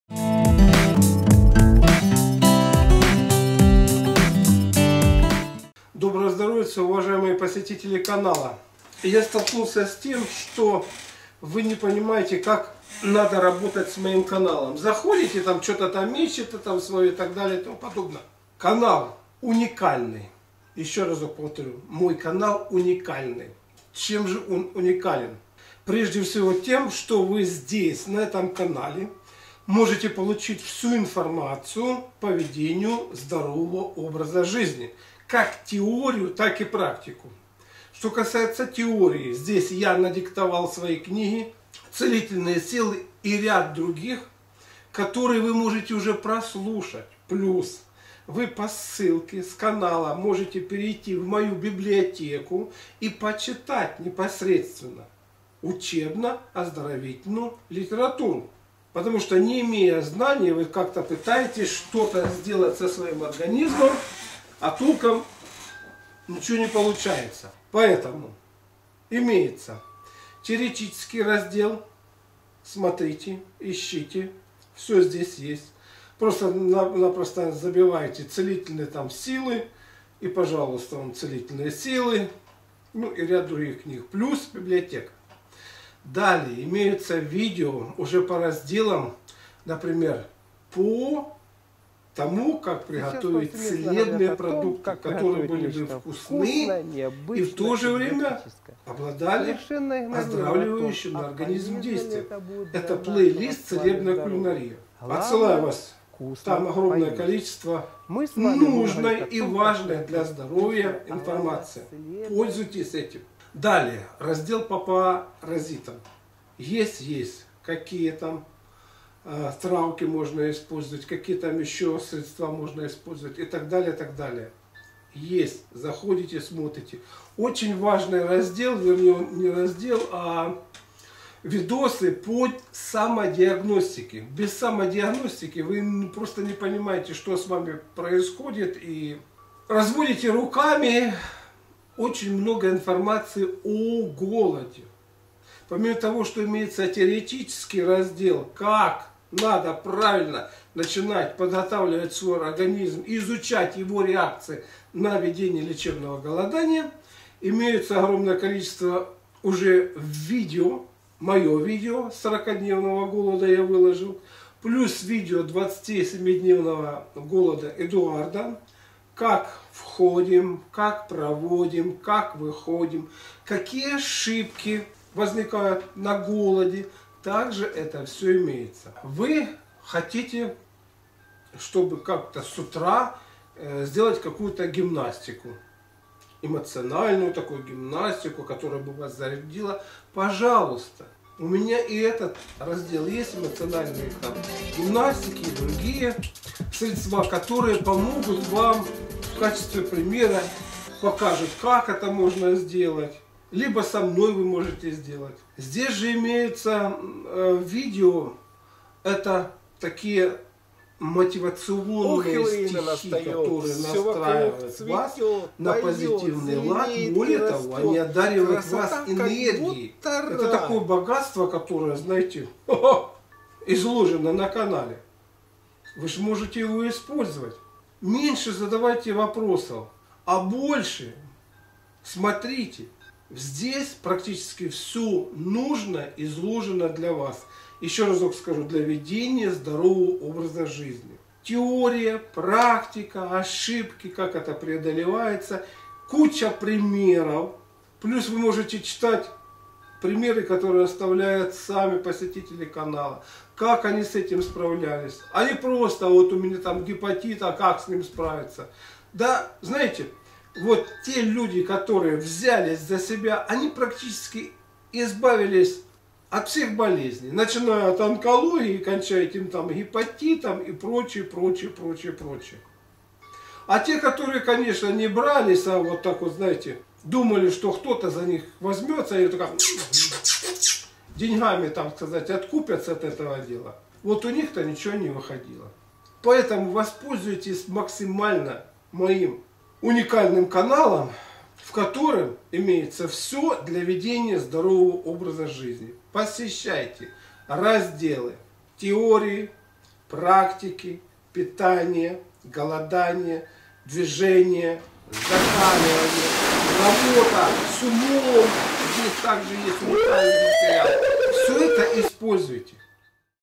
КАК ПОЛЬЗОВАТЬСЯ МОИМ КАНАЛОМ. МАЛАХОВ.
Разъясняю, как правильно пользоваться моим каналом, что бы был хороший результат. Автор идеи и создатель передач, Малахов Геннадий.